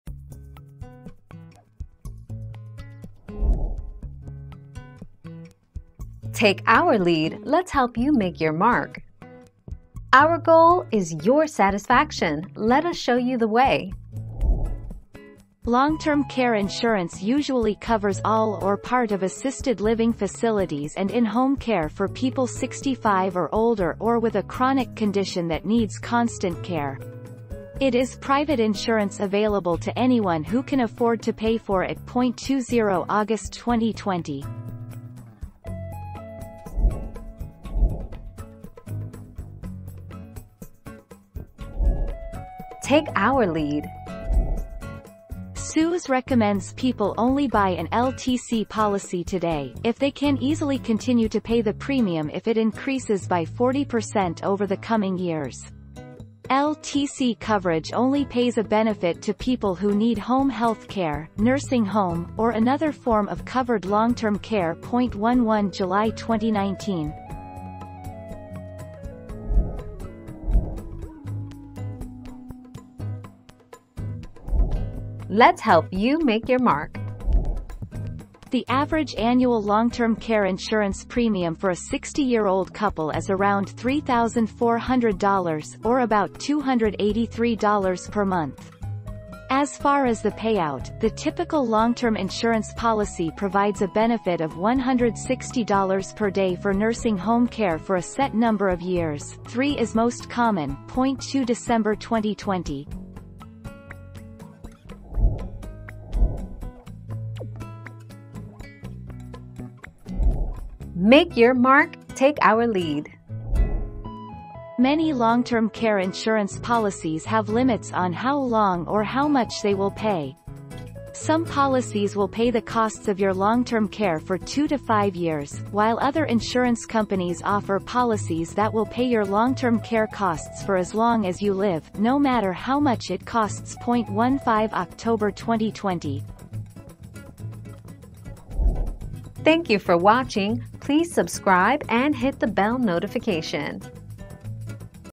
Who would most likely need long term care insurance?
00:00 - Who would most likely need long term care insurance?
00:38 - Does Suze Orman recommend long term care insurance?
01:11 - What is the average monthly cost of long term care insurance?
01:46 - How many years does long term care insurance cover?

Laura S. Harris (2021, March 5.) Who would most likely need long term care insurance?
    AskAbout.video/articles/Who-would-most-likely-need-long-term-care-insurance-225317